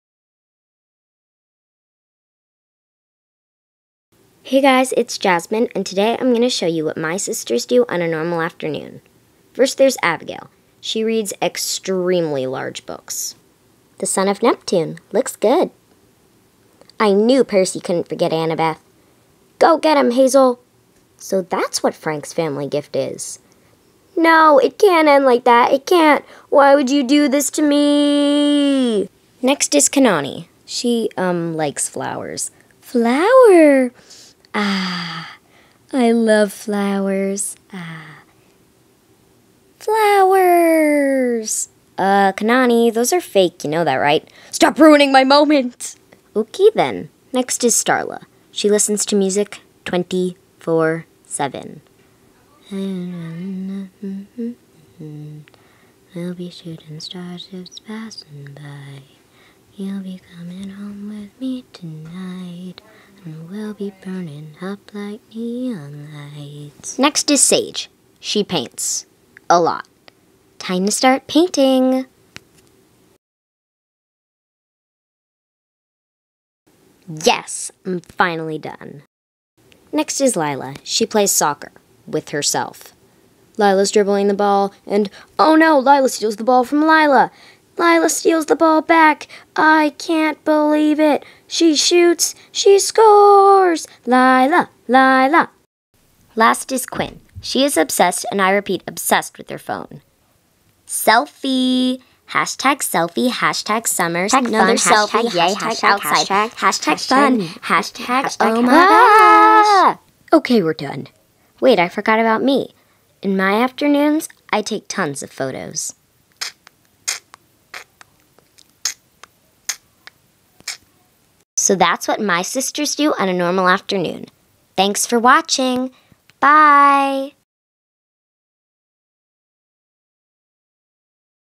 What My Sisters do on a Normal Afternoon AGSM
Hi! It was so nice outside so I decided to make this video. I think it is pretty cute! Who else likes the Percy Jackson series? It is an awesome series. You should read it. (Abigail seems to like it. Bye!

-AmericanGirlRocks